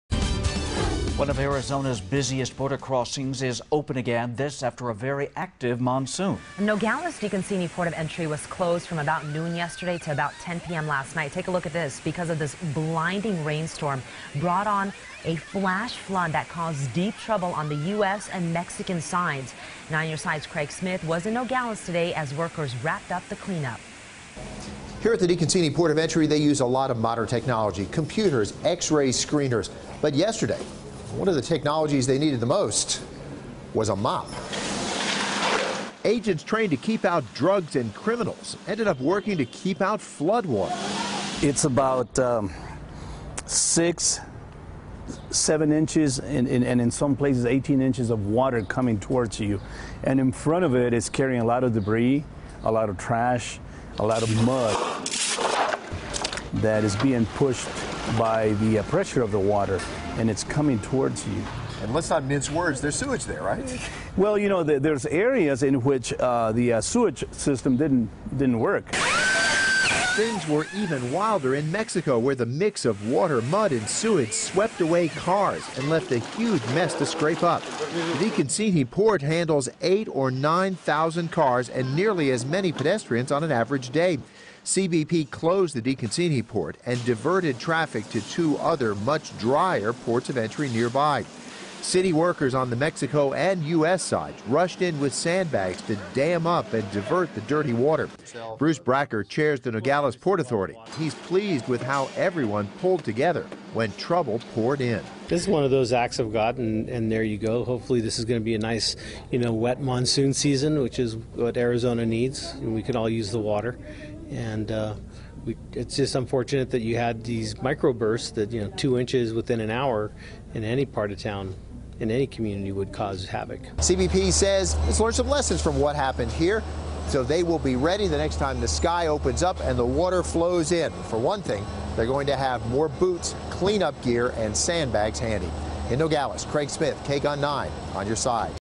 DeConcini Port re-opens after monsoon flood
A flash flood pushed water, mud and sewage into the port